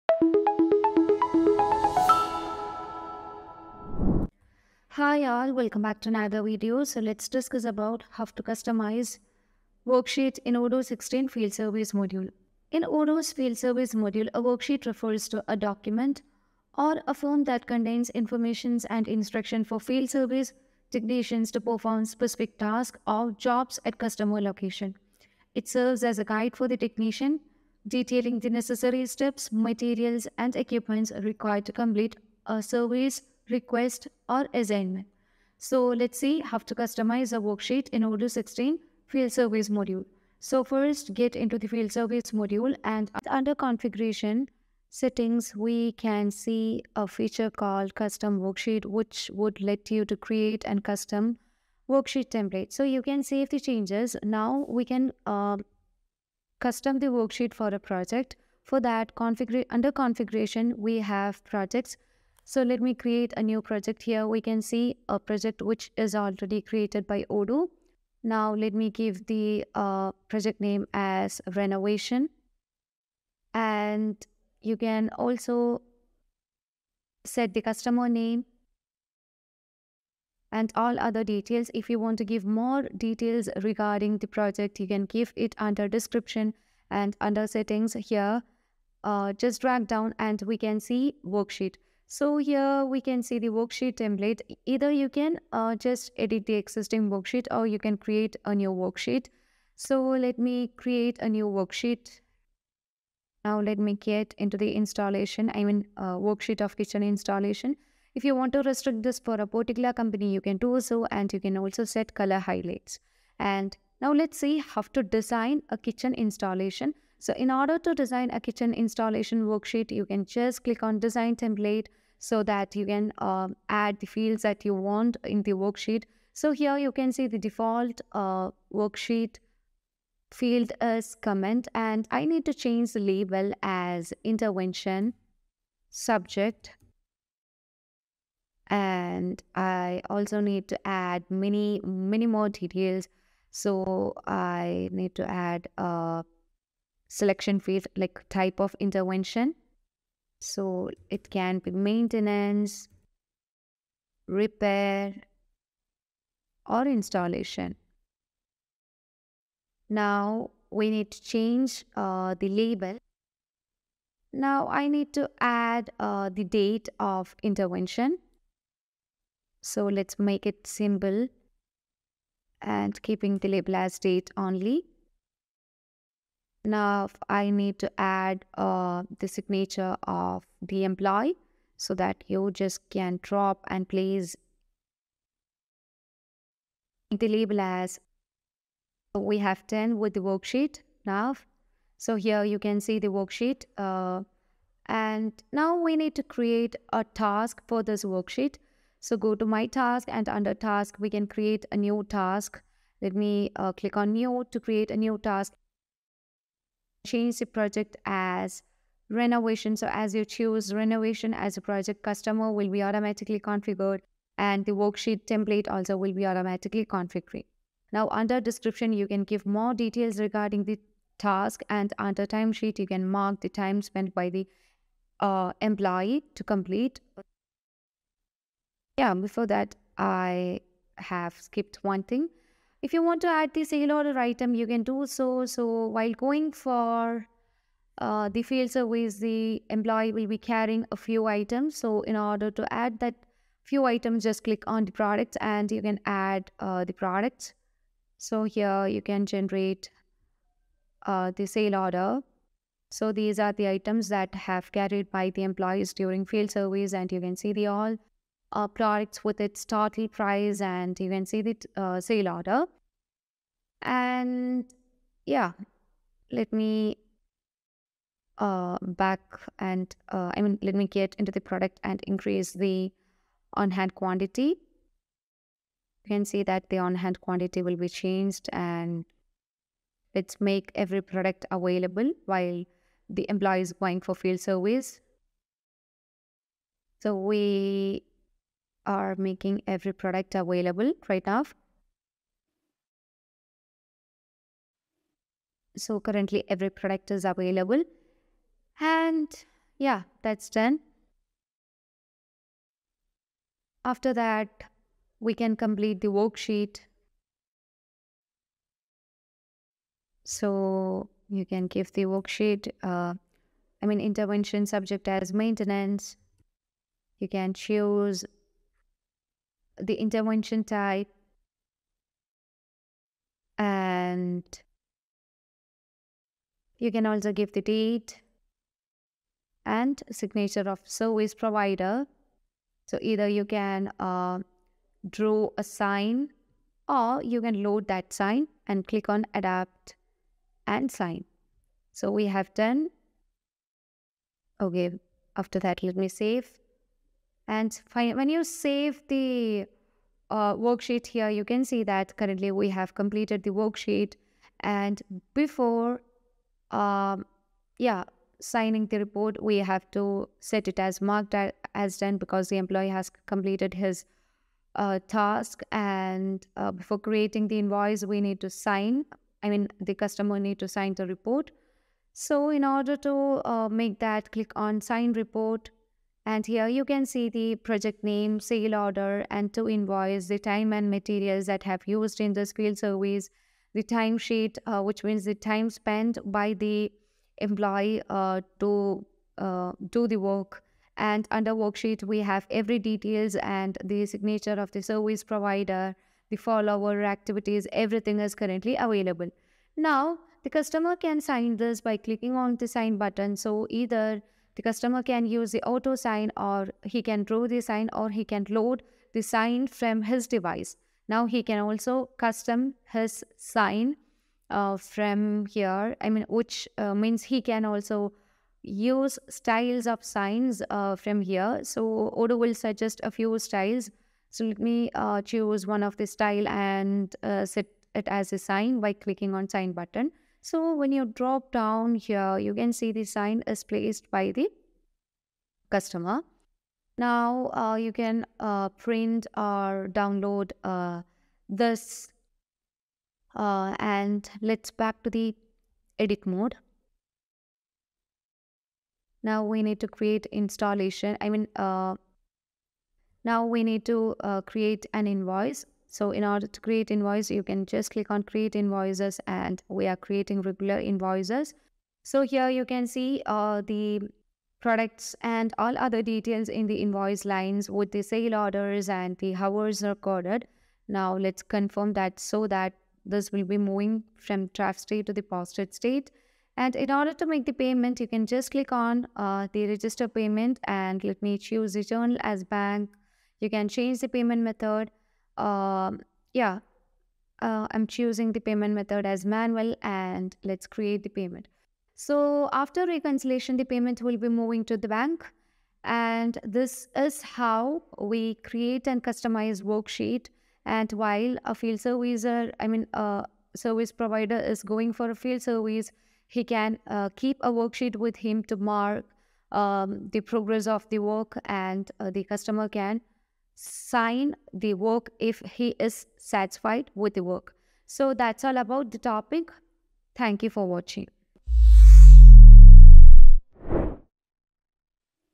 How to Customize Worksheet in Odoo 16 Field Service | Create Worksheet in Odoo 16
This video covers the use and creation of Worksheet Templates in the Field Service application of Odoo 15. Worksheet Templates allow you to create a step in Field Service tasks where you can gather information and collect signatures from workers or clients.

Creating a Worksheet Template
Go to CONFIGURATION - WORKSHEET TEMPLATES. Here you'll see the existing Worksheet Templates. To add another, click CREATE. Fill in the following information:

Name: Enter a name for the Worksheet.
Companies: In a multi-company configuration, select the company(ies) that can use this template. Leave it empty to give access to all companies.
Color: To associate this worksheet with a color, select it here.

When you're done with this initial information, click SAVE.

Worksheet Template Design
Next click DESIGN WORKSHEET TEMPLATE, which was added when you saved the template.

This opens the Studio editor, and we will not go into detail on all of them in this document. However, you are able to drag widgets from the left bar into the worksheet to customize it.

Once you've created your template, click CLOSE. This shows you a preview of the worksheet.

Using Worksheet Templates
Worksheet Templates are assigned to individual tasks using the field of the same name. When applied, you'll see a Worksheet smart button.

Clicking that button will open the worksheet so it can be filled out. Click SAVE when complete.